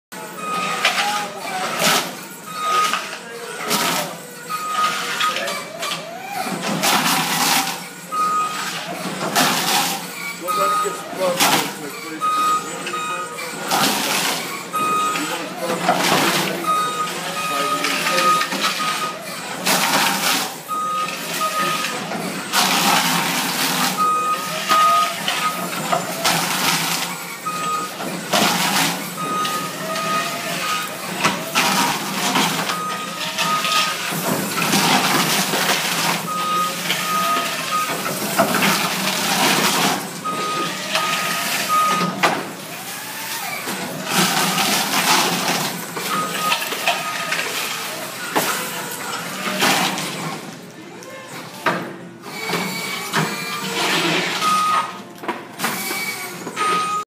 Quarry Tile Removal - Innovatech Terminator T2100EI
Here's the Innovatech Terminator T2100EI ripping through red quarry tile on top of a membrane with ease!